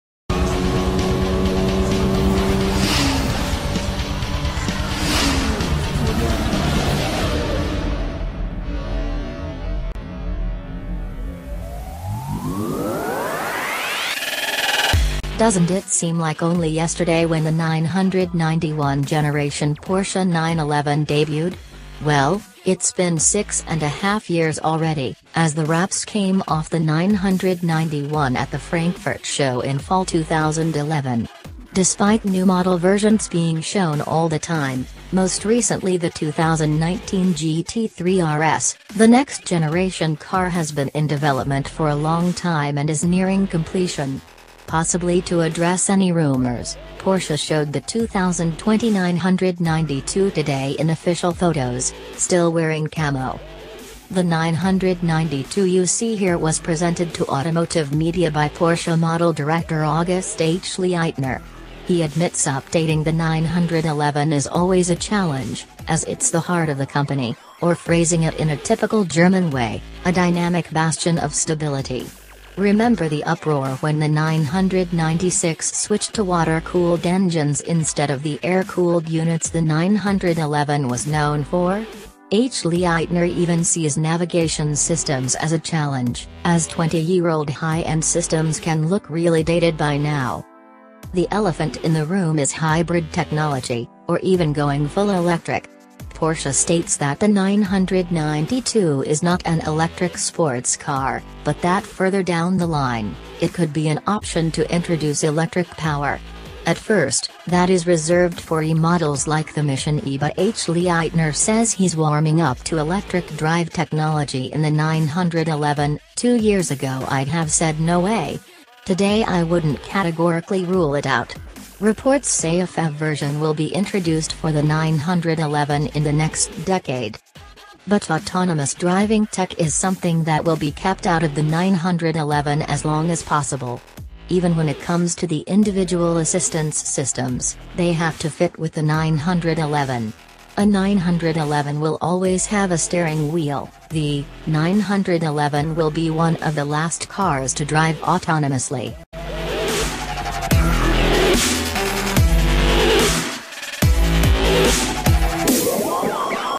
Porsche shows off next gen 911 in camo, reveals some tidbits
Doesn't it seem like only yesterday when the 991 generation Porsche 911 debuted? Well, it's been 6½ years already, as the wraps came off the 991 at the Frankfurt show in fall 2011. Despite new model versions being shown all the time, most recently the 2019 GT3 RS, the next-generation car has been in development for a long time and is nearing completion. Possibly to address any rumors, Porsche showed the 2020 992 today in official photos, still wearing camo.

The 992 you see here was presented to automotive media by Porsche model director August Achleitner. He admits updating the 911 is always a challenge, as it's the "heart of the company," or phrasing it in a typical German way, "a dynamic bastion of stability." Remember the uproar when the 996 switched to water-cooled engines instead of the air-cooled units the 911 was known for? Achleitner even sees navigation systems as a challenge, as 20-year-old high-end systems can look really dated by now.

The elephant in the room is hybrid technology, or even going full electric. Porsche states that the 992 is "not an electric sports car," but that further down the line, it could be an option to introduce electric power. At first, that is reserved for "e-models" like the Mission E. But Achleitner says he's warming up to electric drive technology in the 911: "Two years ago I'd have said no way. Today I wouldn't categorically rule it out." Reports say a PHEV version will be introduced for the 911 in the next decade.

But autonomous driving tech is something that will be kept out of the 911 as long as possible. "Even when it comes to the individual assistance systems, they have to fit with the 911. A 911 will always have a steering wheel, [the] 911 will be one of the last cars to drive autonomously."